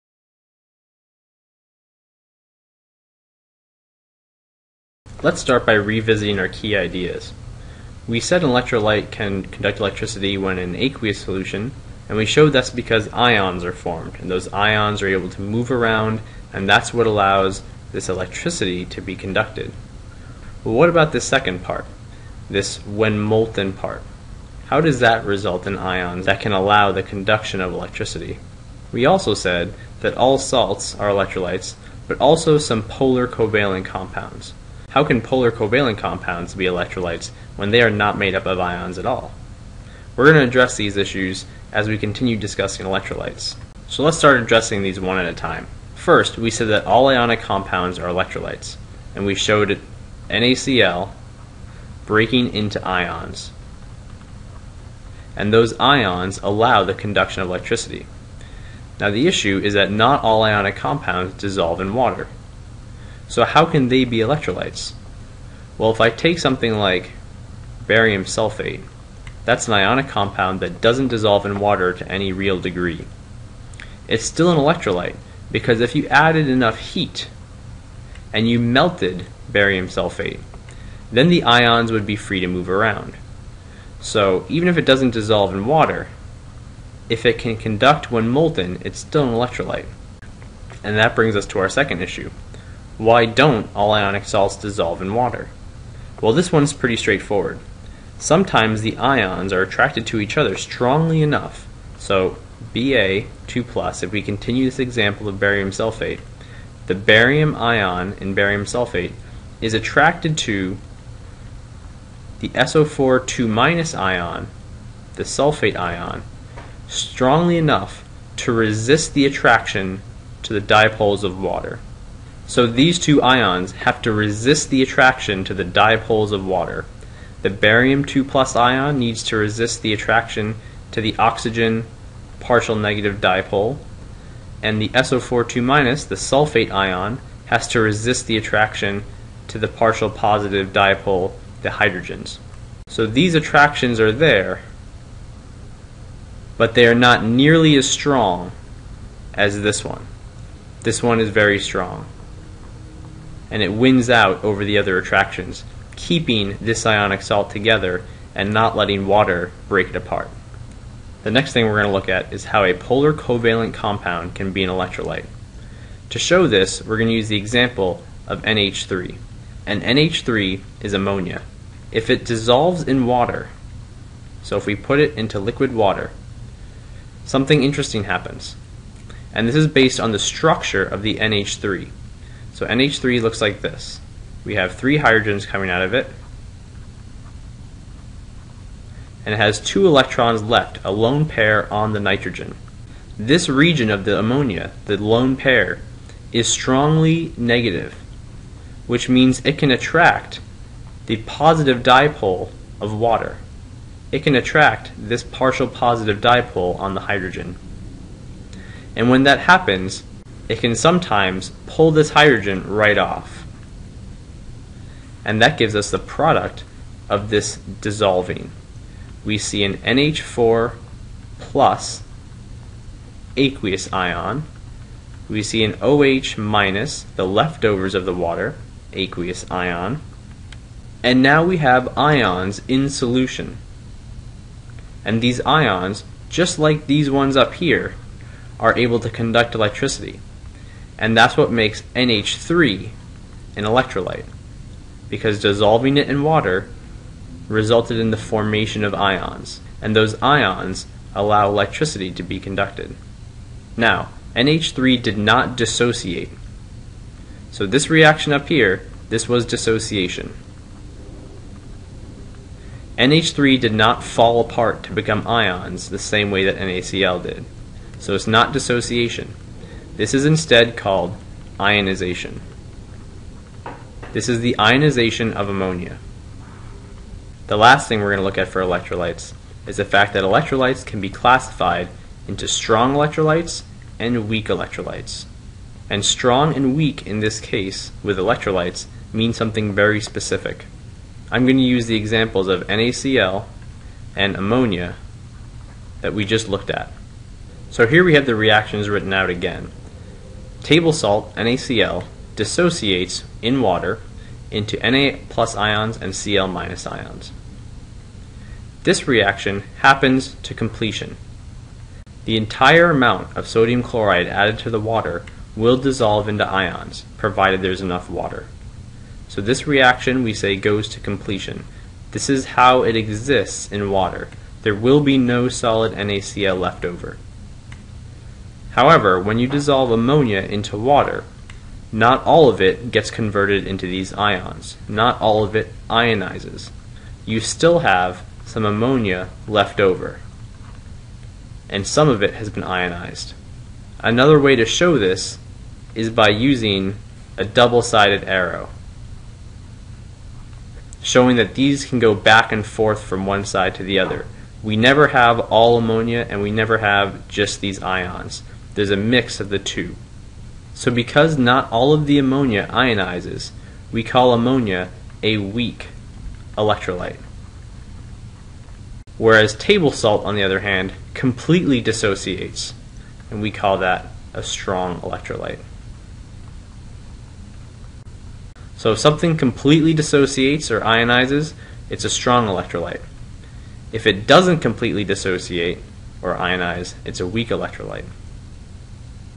Chemistry 9.2b More on Electrolytes
This lesson goes into greater detail about electrolytes, including the difference between ionic compounds and polar covalent electrolytes. It also distinguishes between dissociation and ionization, and discusses Strong vs Weak. electrolytes.